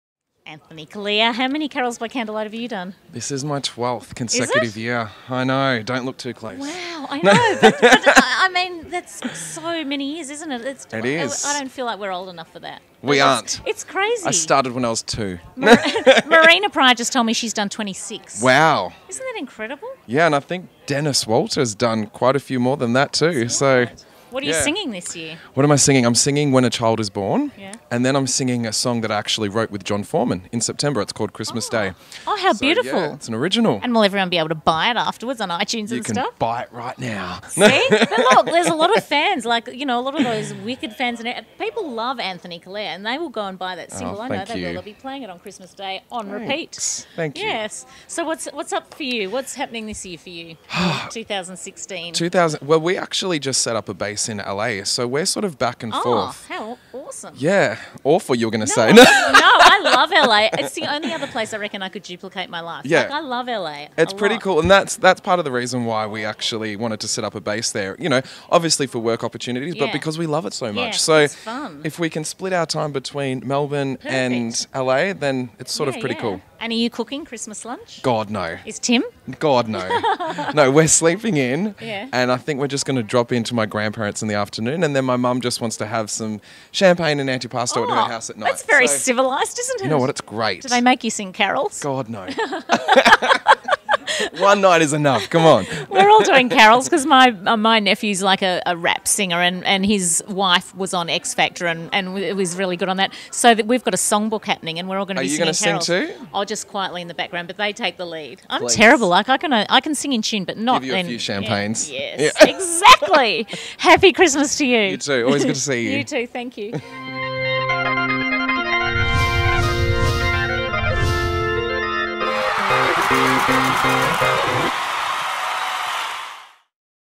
ANTHONY CALLEA IS SINGING A NEW SONG ON CAROLS BY CANDLELIGHT 2015
I've ALWAYS loved Anthony Callea's voice from all those years ago when he was on Australian Idol and now here I am chatting to him backstage at the rehearsals for Carols By Candlelight. I can't wait to hear his new Christmas song on Carols By Candlelight tonight ‪#‎carolsbycandlelight‬